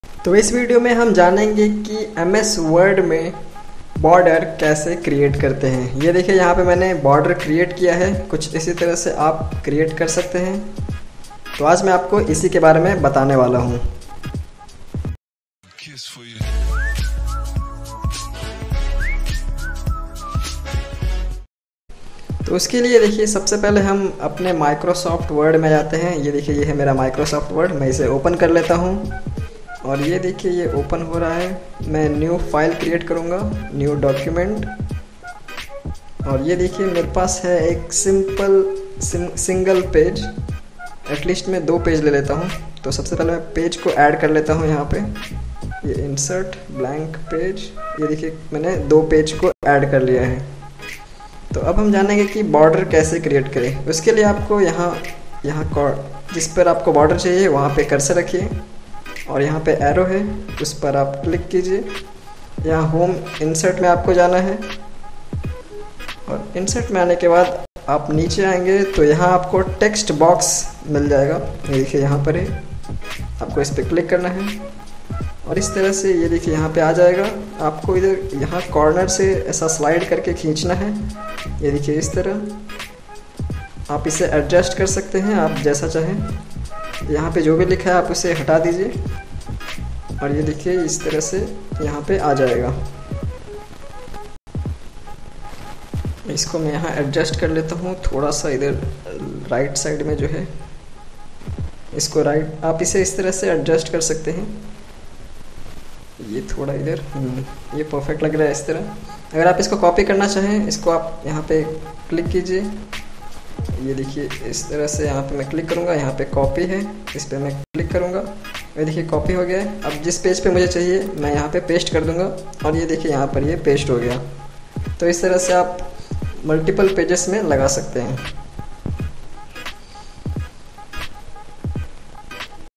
How to create border in ms word in mobile/ ms word me border kaise lagaye
In this video i have told about how to create border in ms word in mobile